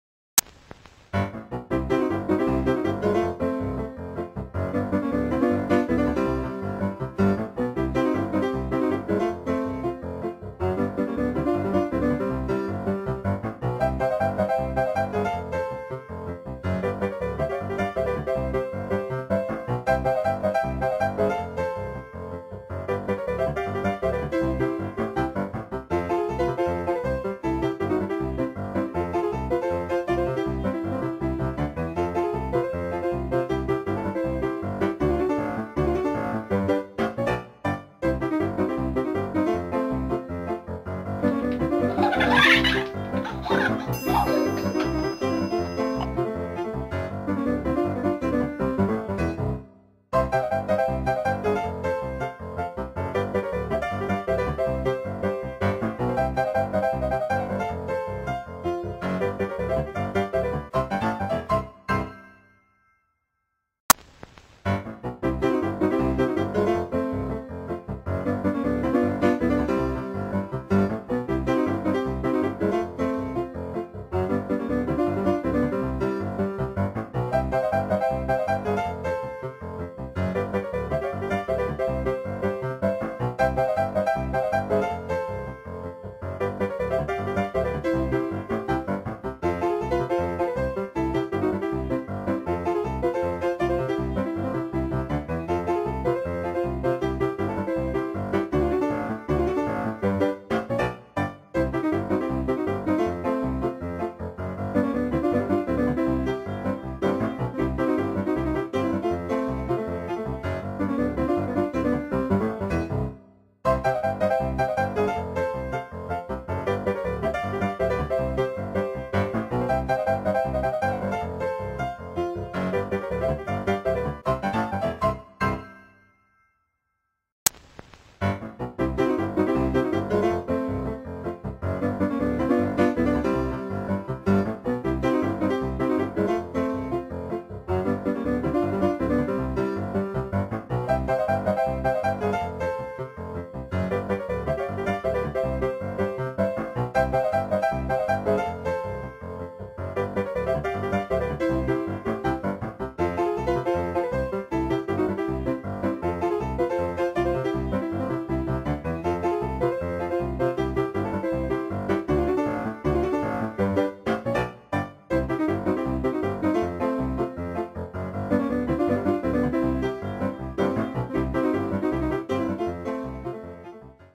DS Poškoláci - táborový den ve stylu Groteska
Video z tábora Před oponou za oponou 2016, shrnutí našeho táborového dne ve stylu Groteska